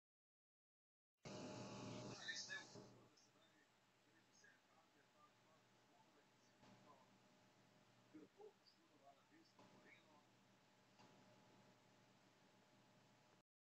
LINDSEY VONN HORRIBLE CRASH (Schladming, 5.2.2013)
Auch!